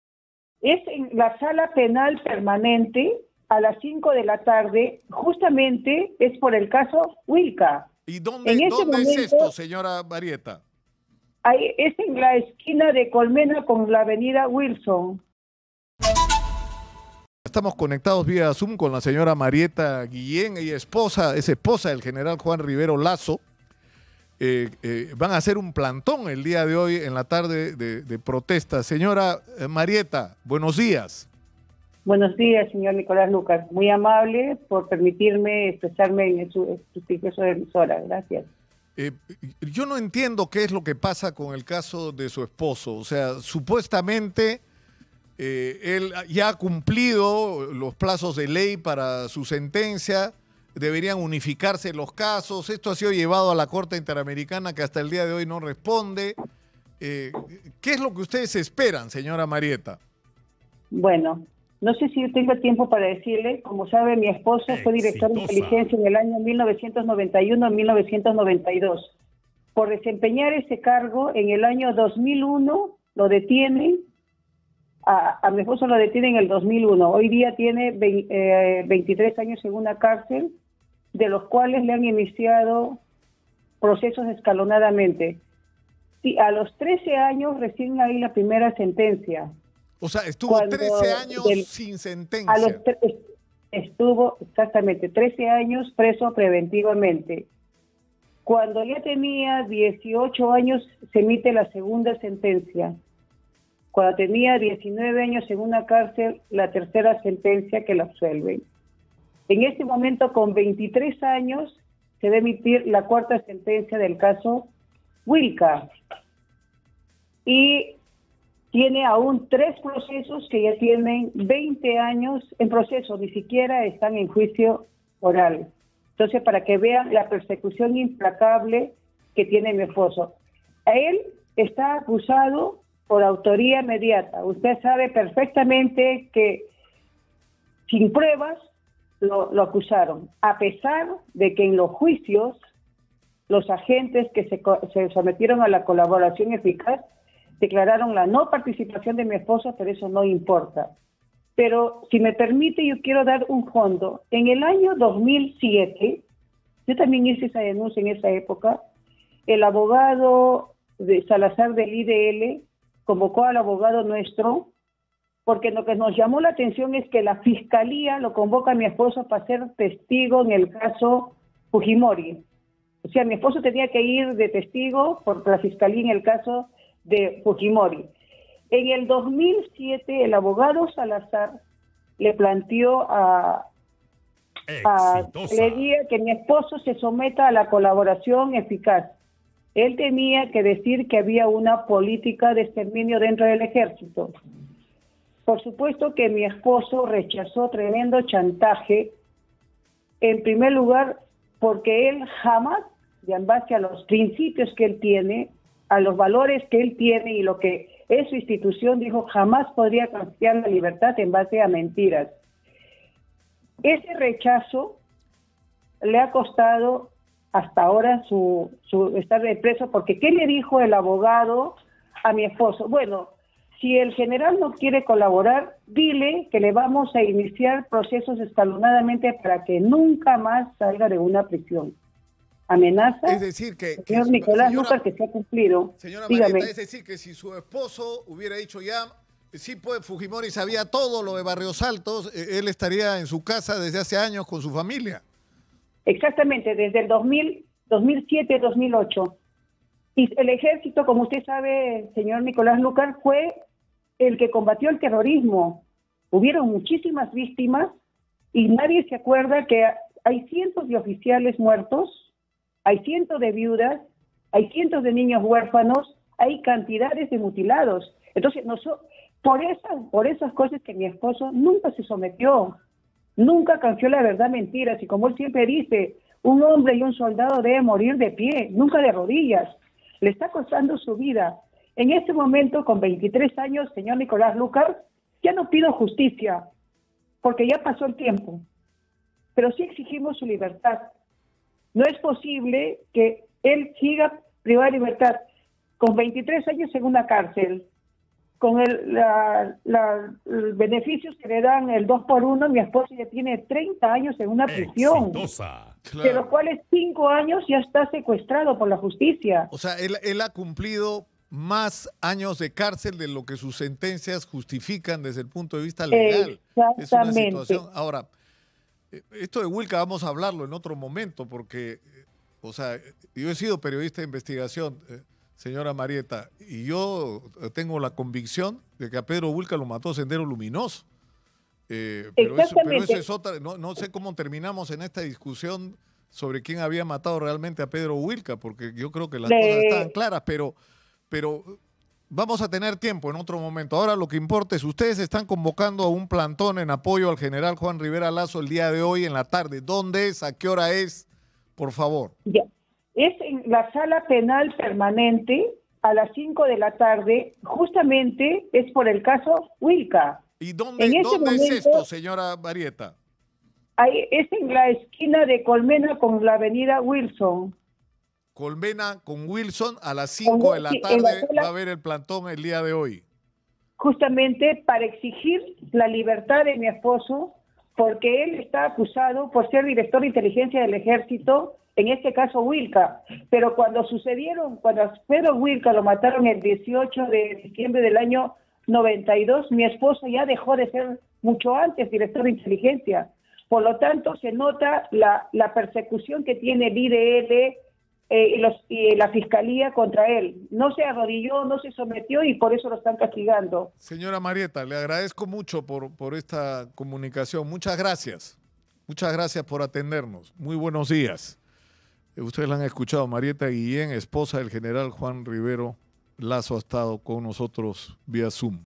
🔴🔵Caso Pedro Huilca: Convocan a un plantón para exigir la libertad de Juan Rivero Lazo
Convocan a un plantón para exigir la libertad de Juan Rivero Lazo, exdirector de Inteligencia del Ejército, por el caso Pedro Huilca.

Exitosa Perú
"Exitosa: La voz que integra al Perú"

Corporación Universal